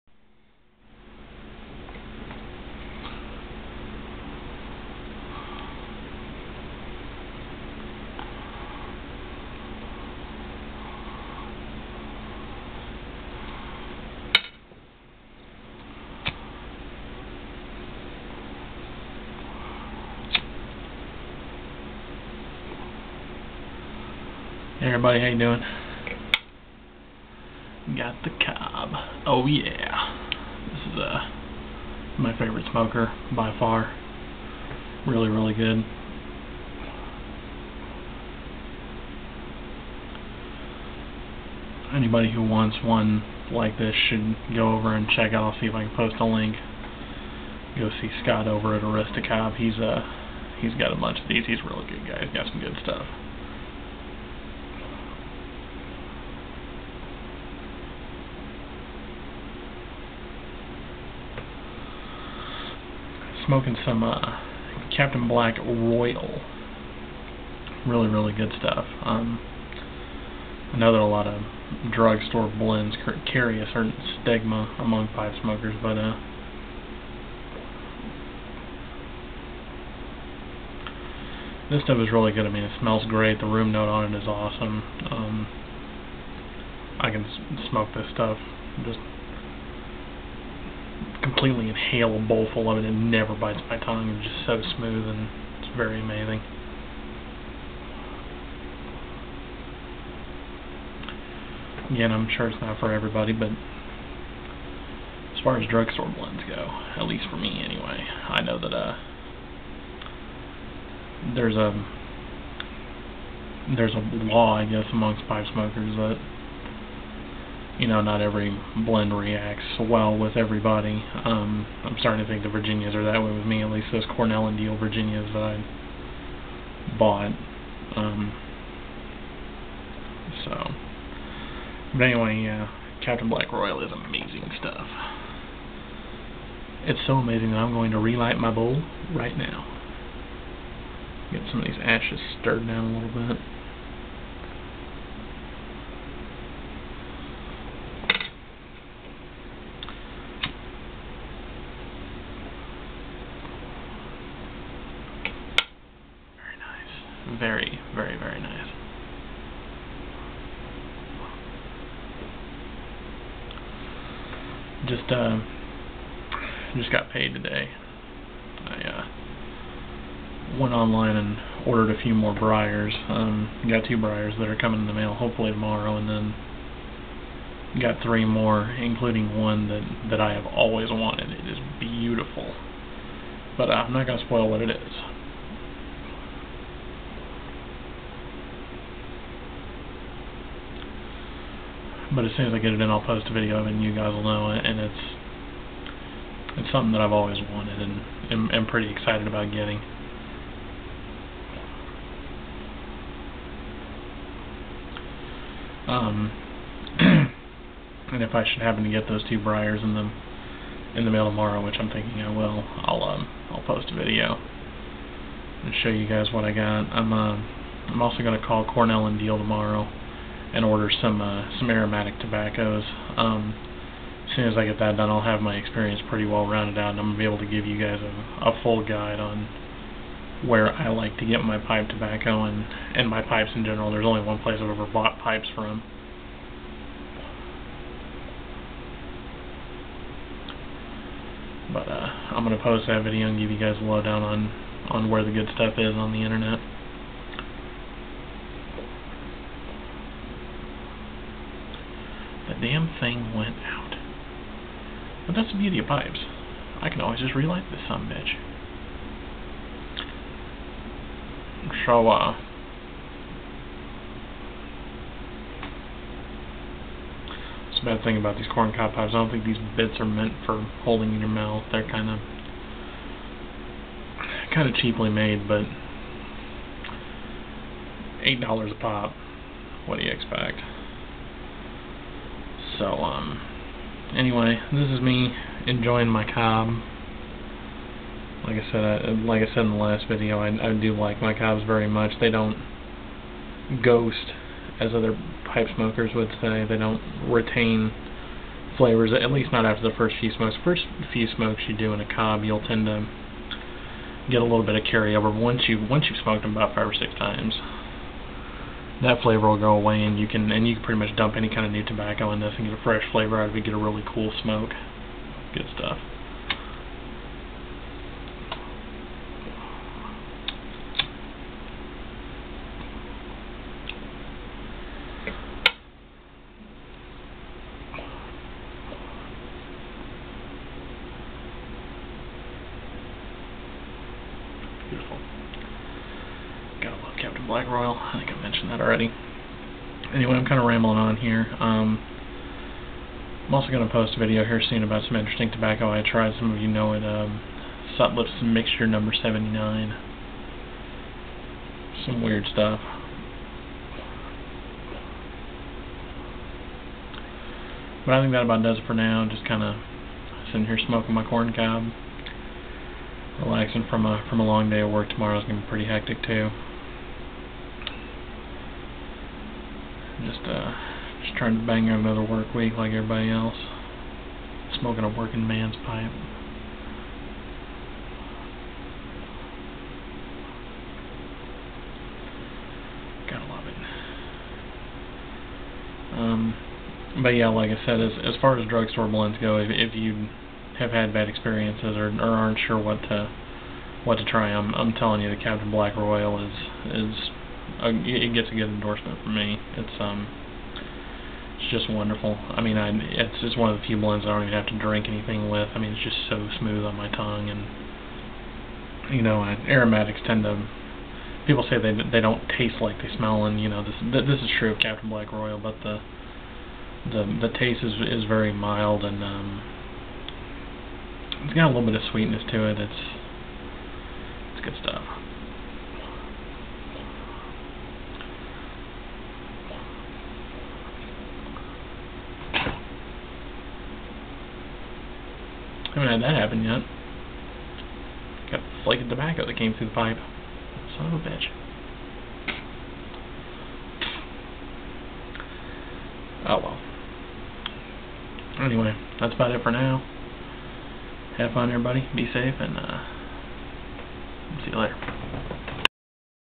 A Working Mans Pipe!
After a hard day at the office, I like to smoke a pipe fit for a hard working man like myself. I also talk about a favorite tobacco and some pipes that you may be seeing before too long. Enjoy!